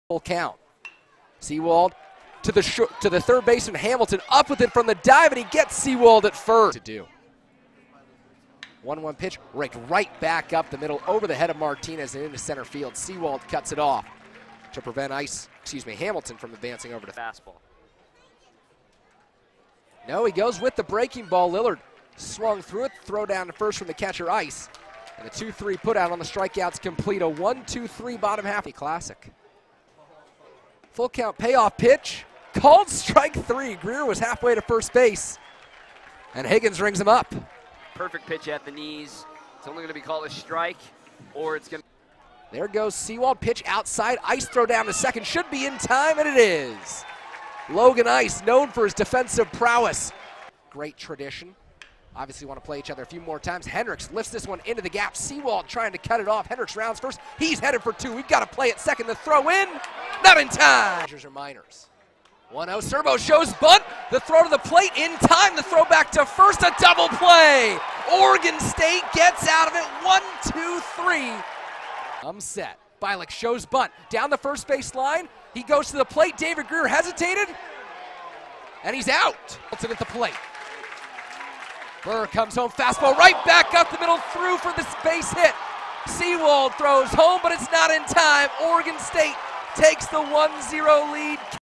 Beavers Win 1-0 To Open Pac-12 Play
Oregon State defeated Arizona State, 1-0, on Friday, March 13, 2015, in Phoenix, Ariz.